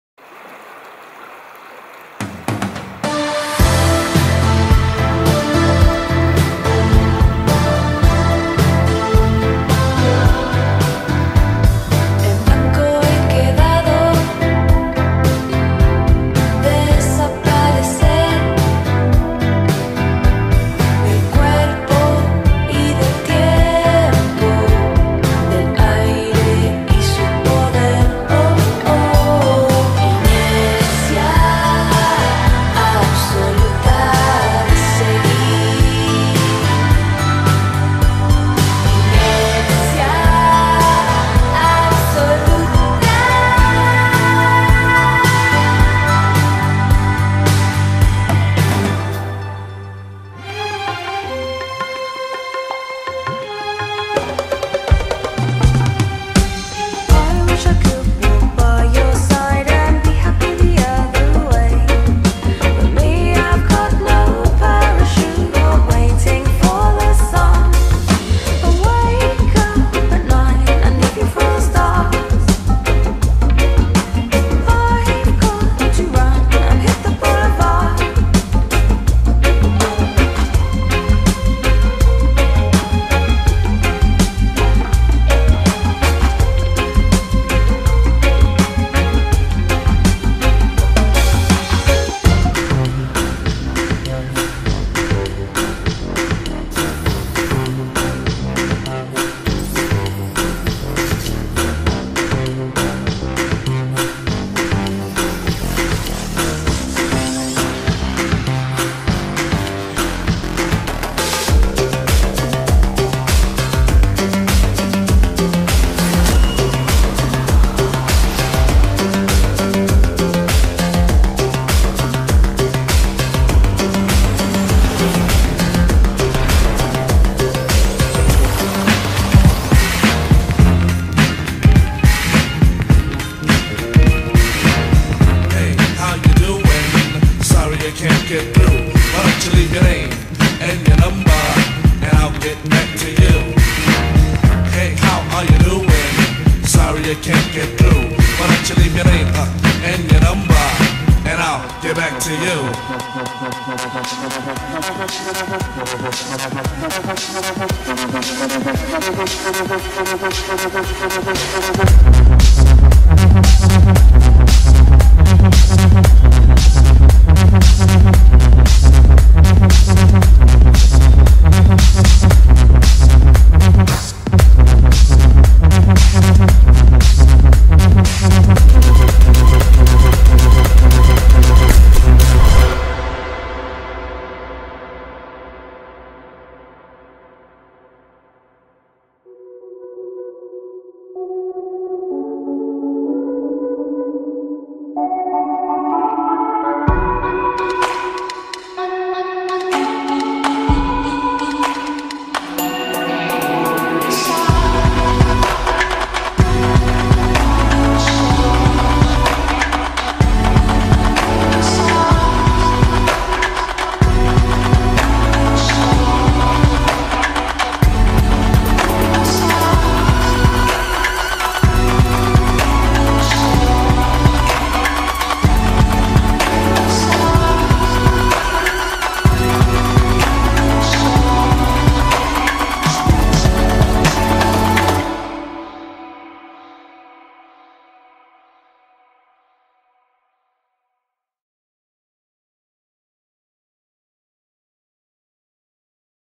Carnaval de Bahidorá 2015
En Indie Rocks! hicimos un recuento de lo que se vivió en el Carnaval de Bahidorá 2015. Revive algunos de los mejores momentos, en tan sólo 4 minutos.